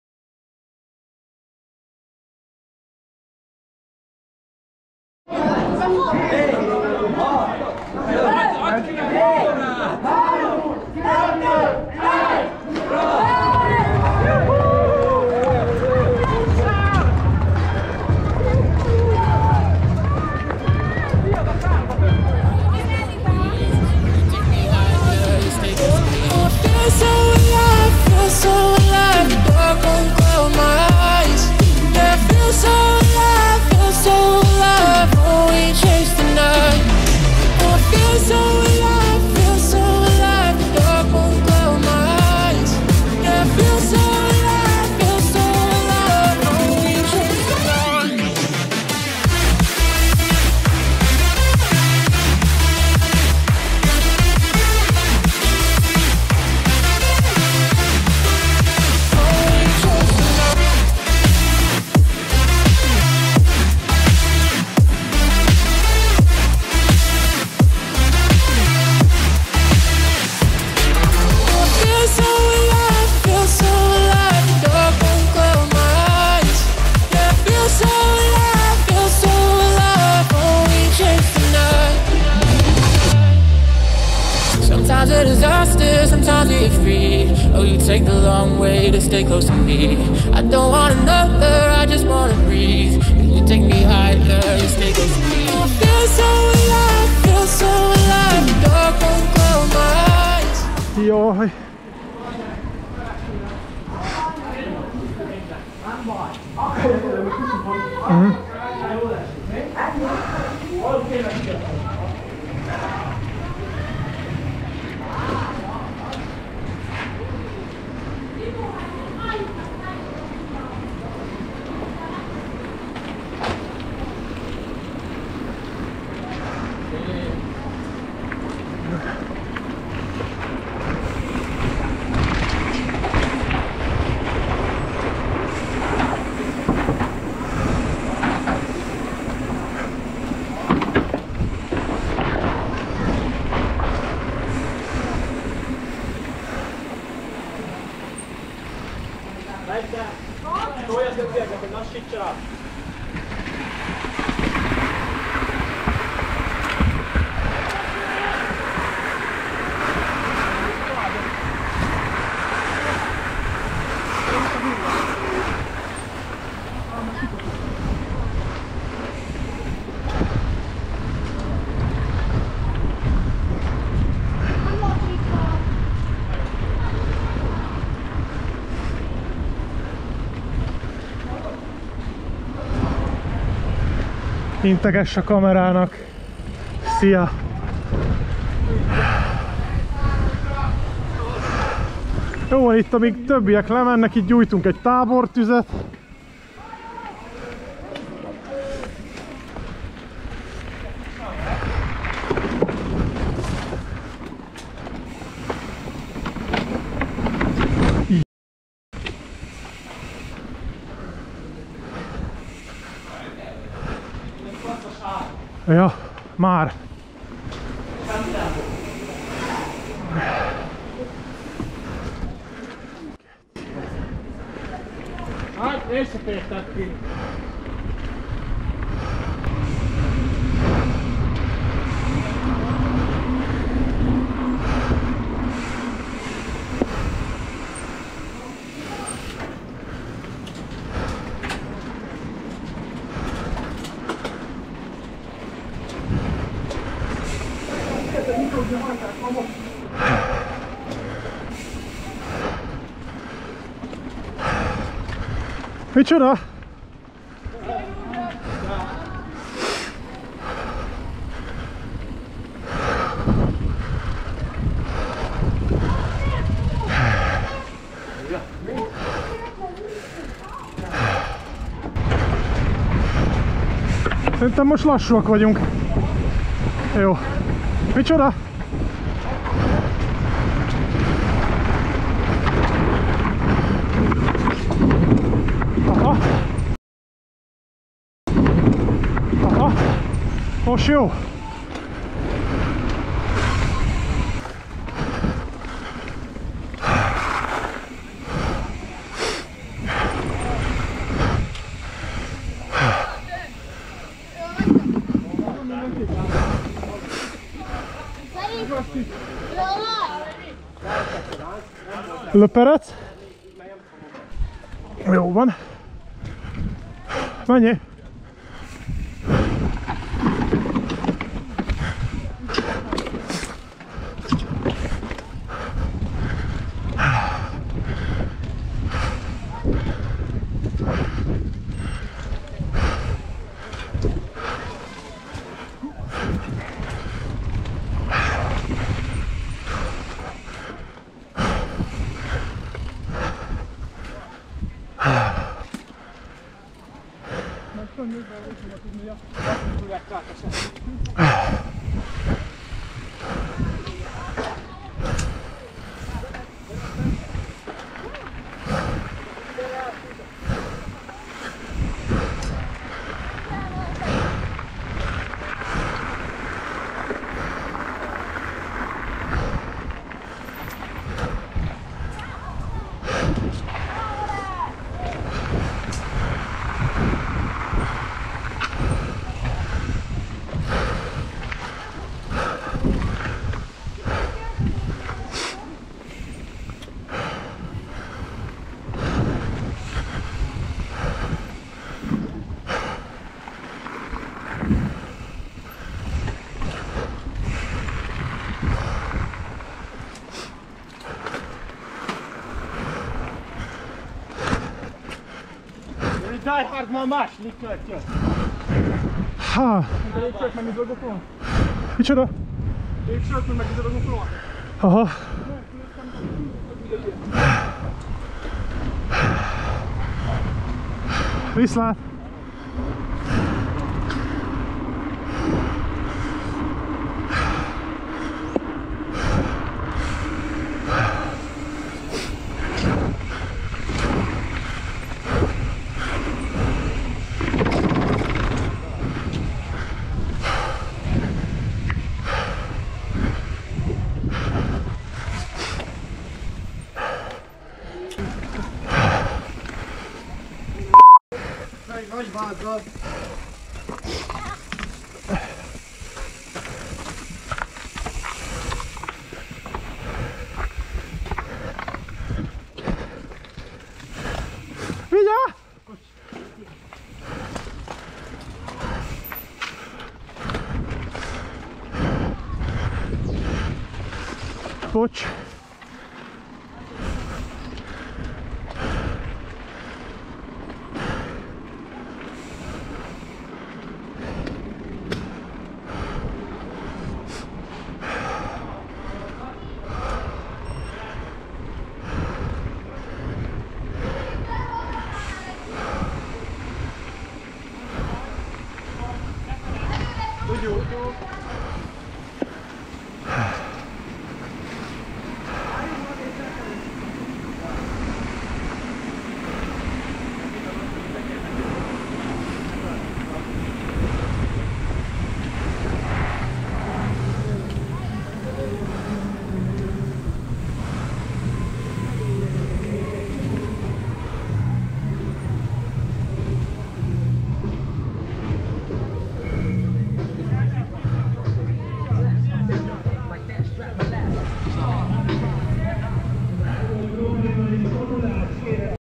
14.METRINWEST CITYCROSS KŐSZEG
első kör a 2022es citycrosson, felesleges beszéd és a feltorlódások nélkül.